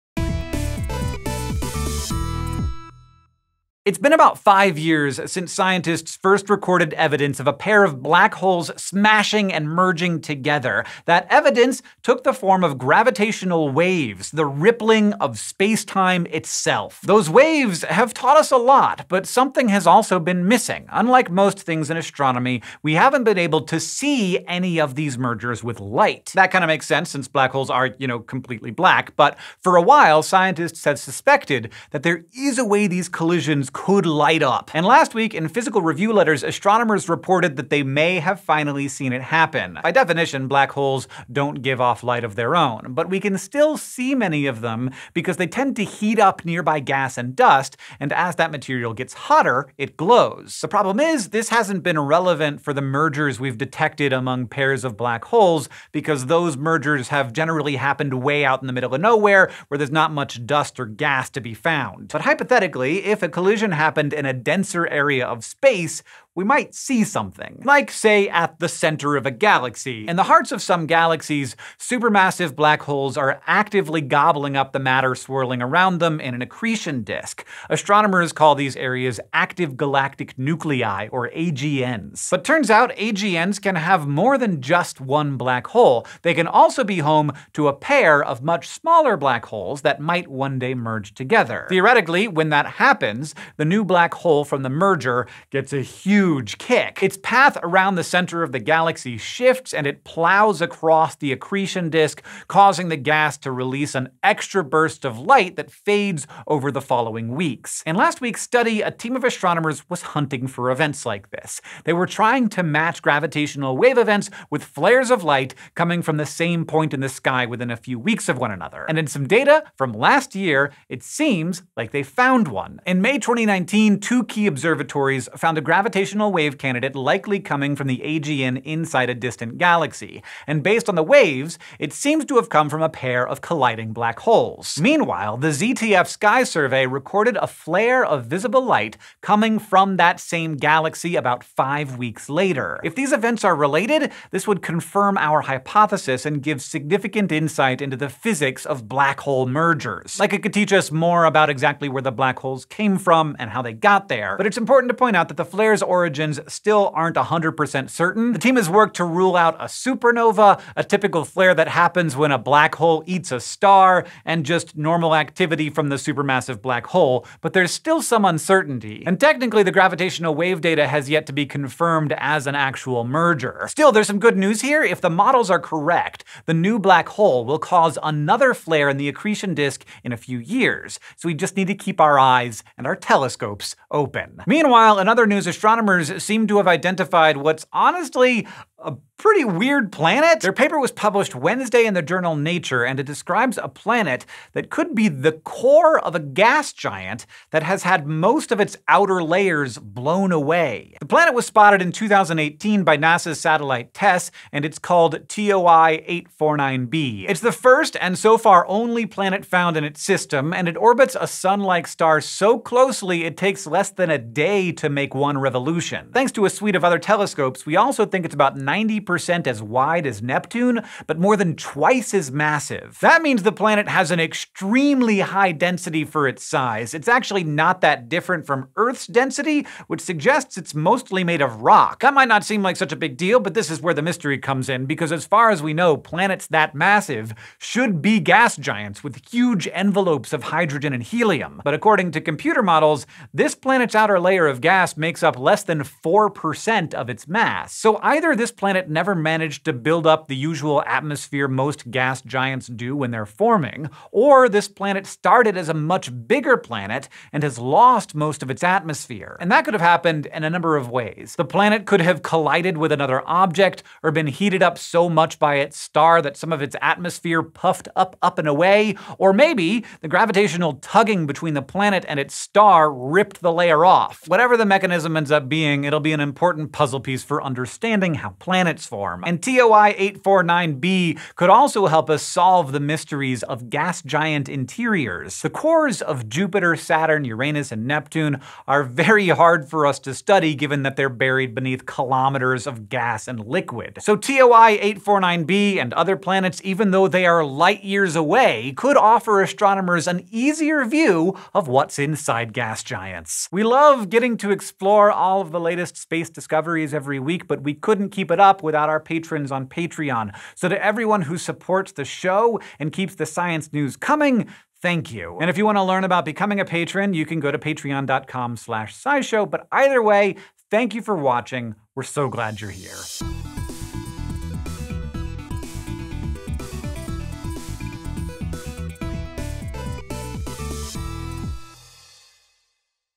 This Planet Used to Be the Core of a Gas Giant? | SciShow News
Scientists may have found the light from two merging black holes, and a gas giant, without the gas.